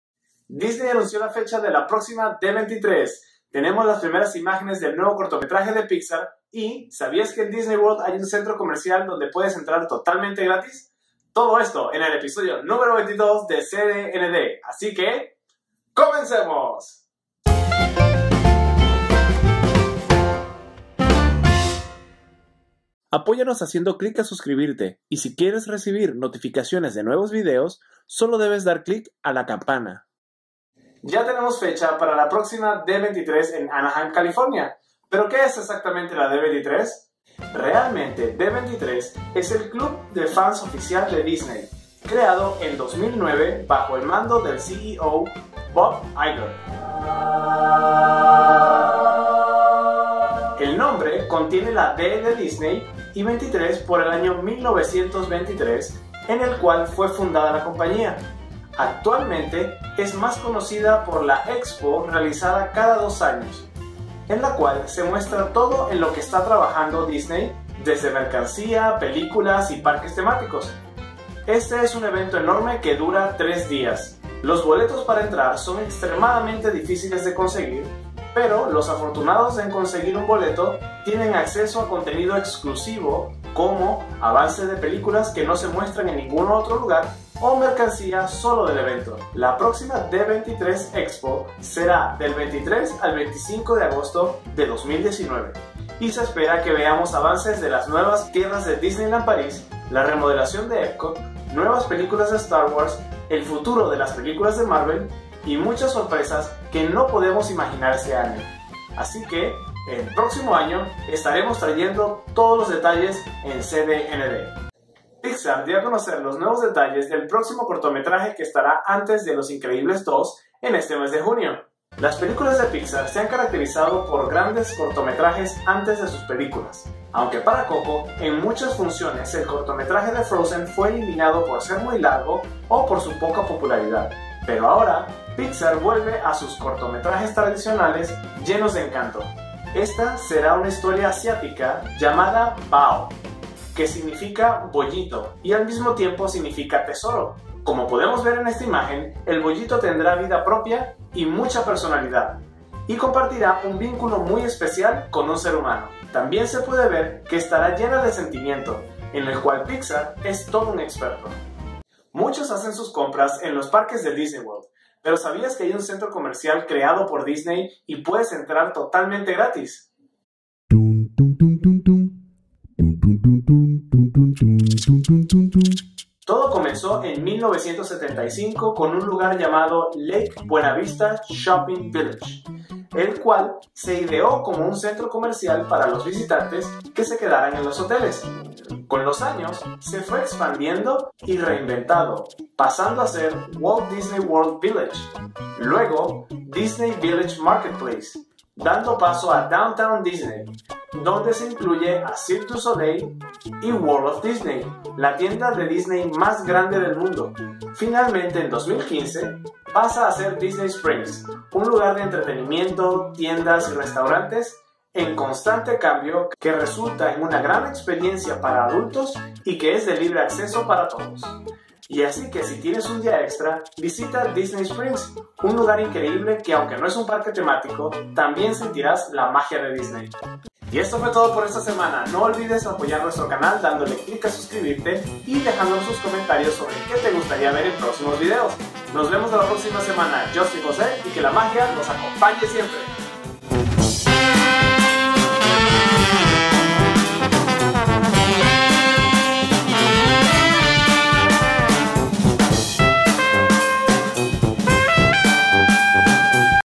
Nuevo Cortometraje de Pixar | Fecha de la D23 2019!
Esta semana hablaremos de la nueva D23, el nuevo cortometraje de Pixar que estará junto a Los Increíbles 2 y sobre Disney Springs, no te lo pierdas.